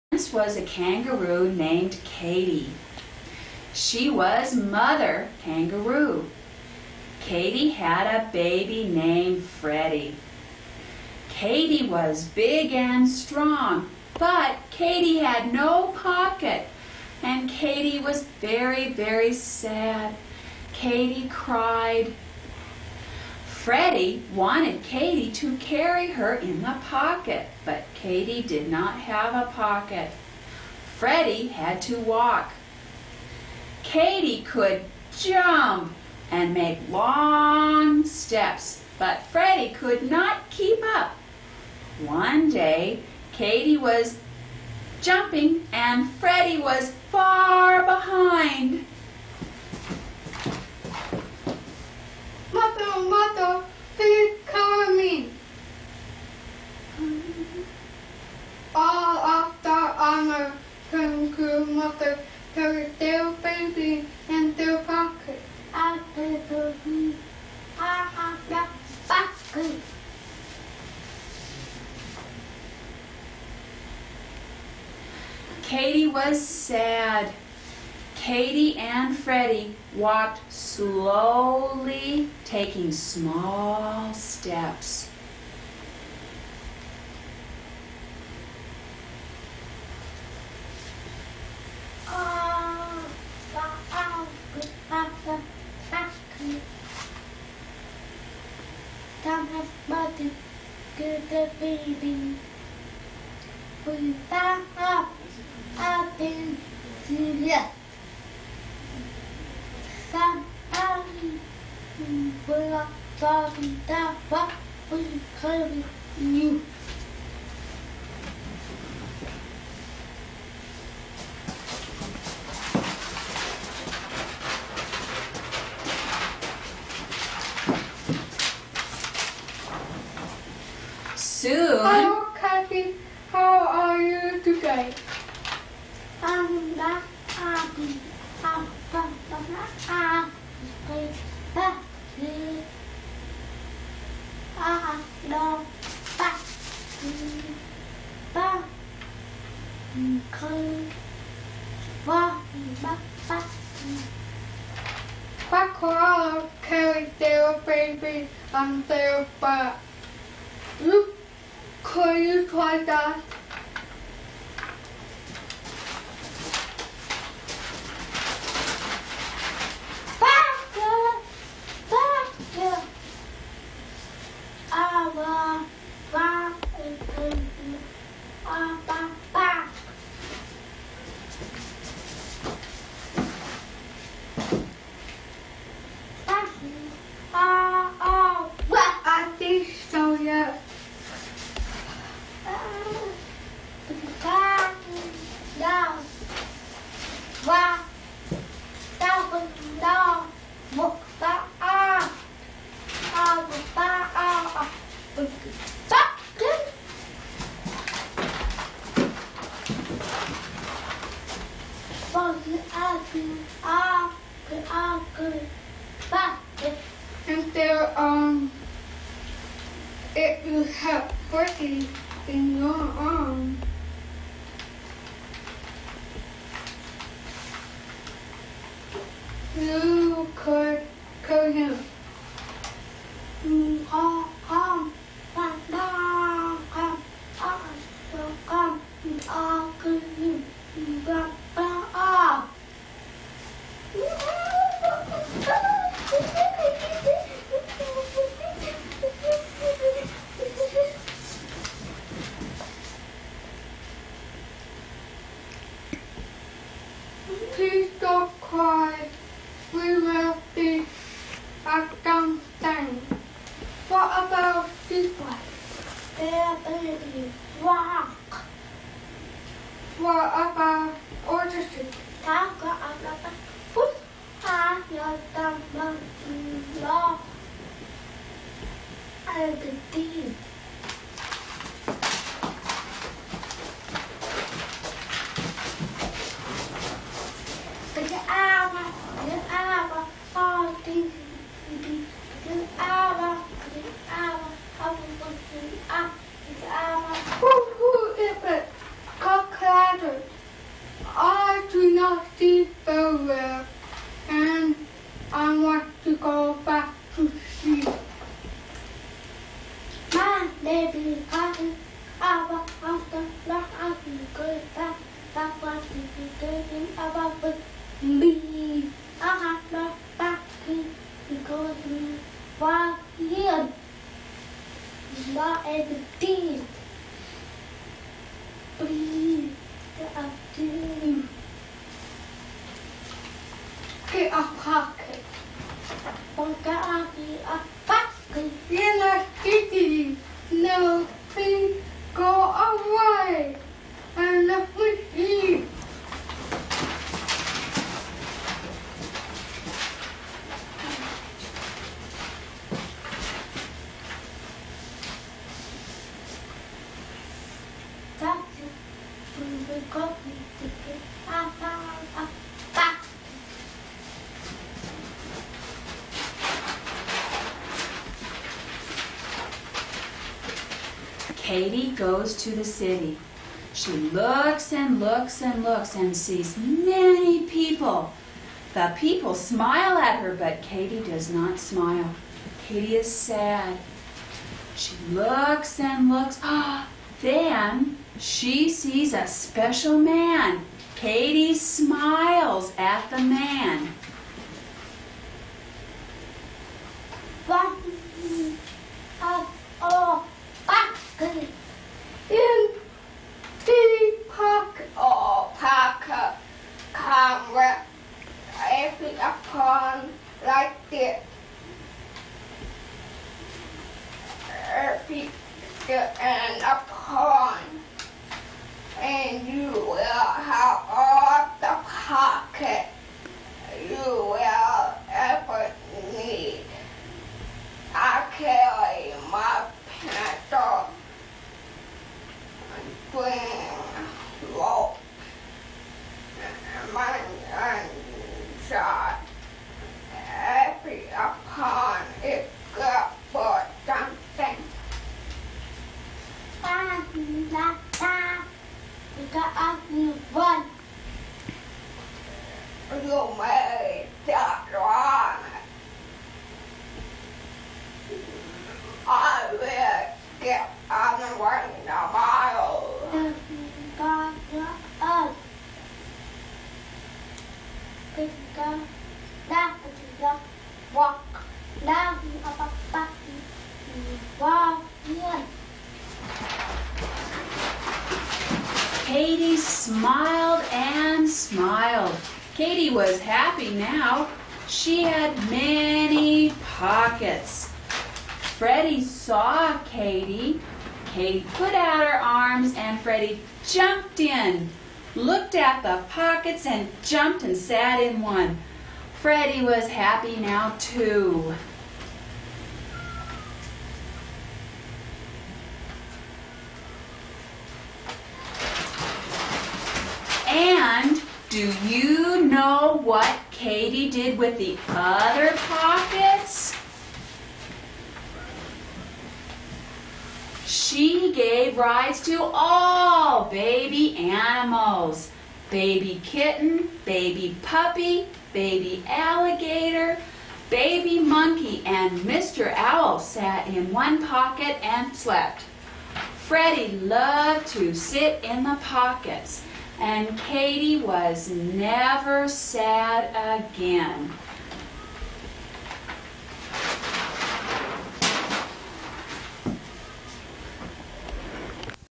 Katy No Pocket - 1991
A class performance of Katy No Pocket.
Filmed by the Wyoming School for the Deaf - May 10, 1991.
Made available by the Wyoming Department of Education.